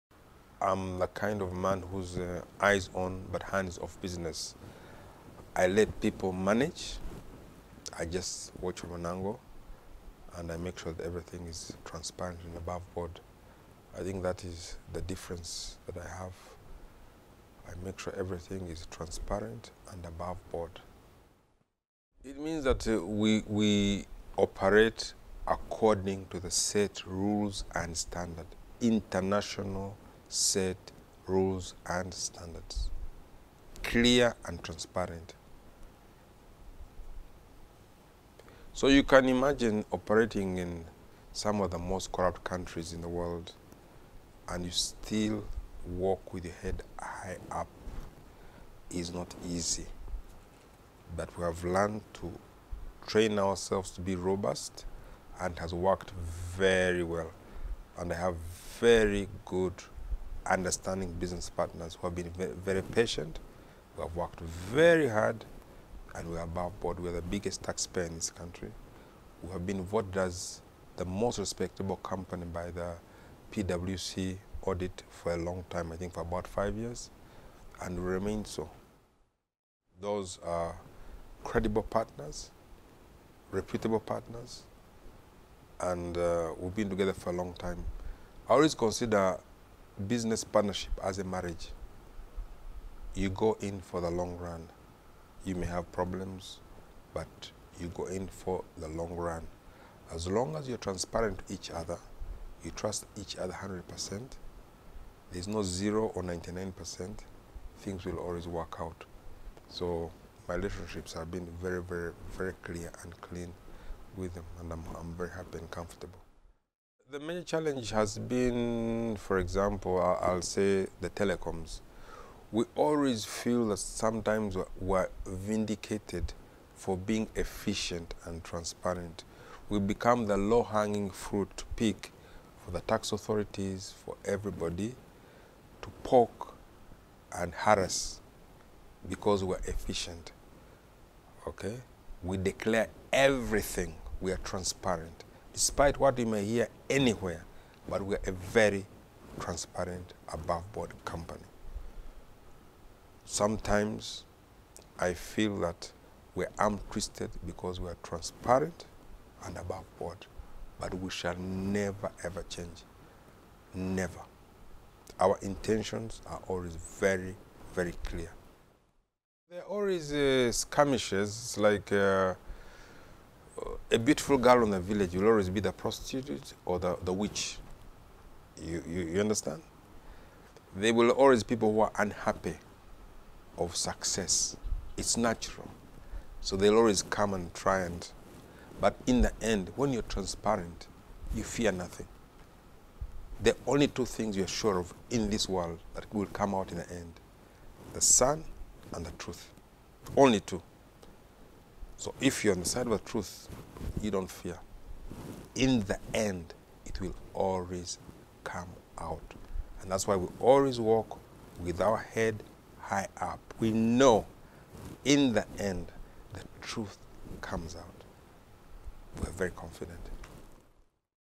The Importance of Doing Business in a Clear and Transparent Way by Business Leader Charles Mbire
Charles Mbire, Chairman of MTN Uganda, talks about the importance of doing business in a transparent and above board way, and explains what are the challenges implied.